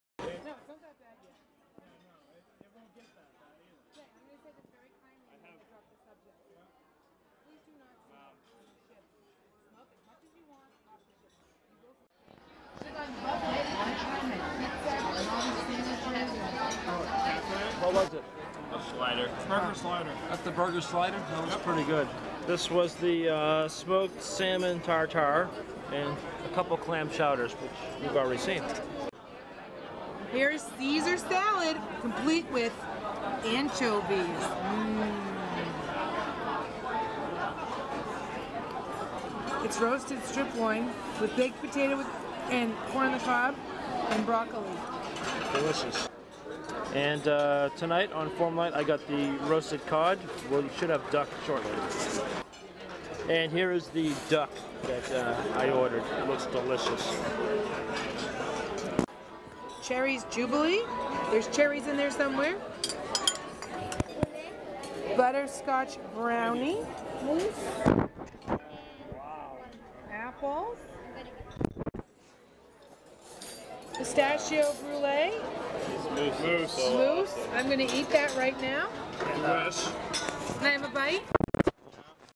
Norwegian Dawn2
Dinner on night two of our cruise to Bermuda. A little more formal attire than other nights but the food was still good. Take a look.